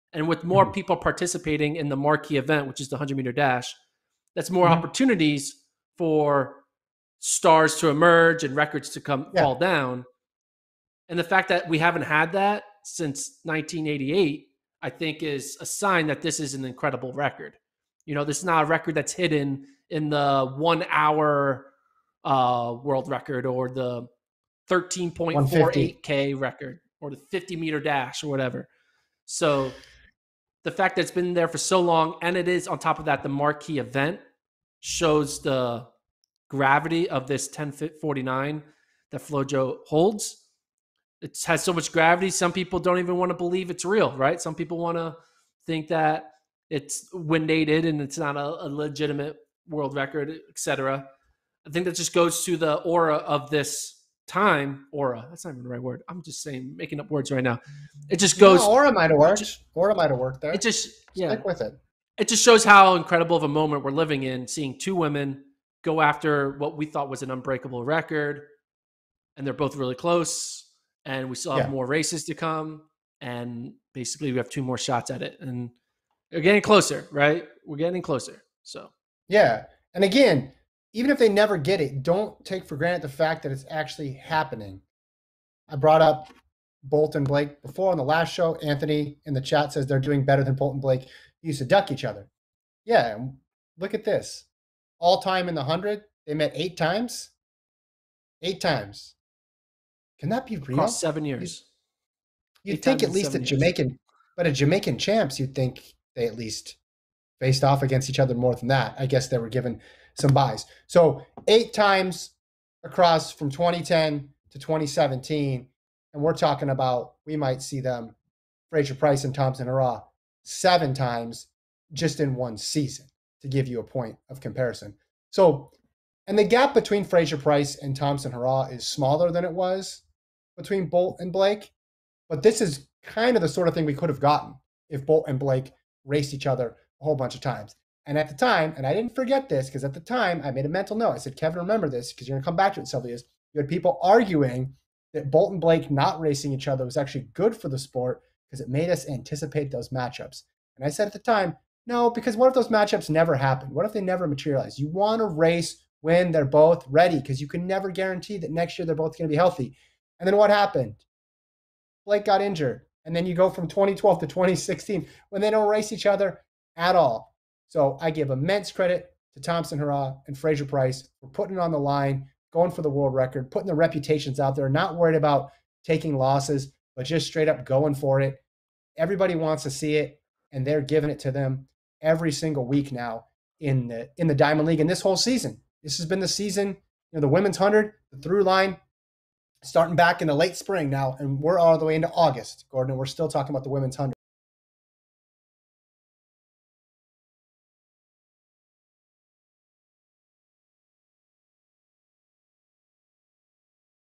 Fraser-Pryce vs Thompson-Herah Is Better Than Bolt vs Blake
Watching Shelly-Ann Fraser-Pryce and Elaine Thompson-Herah battle this season has been more exciting than when Usain Bolt and Yohan Blake avoided racing each other.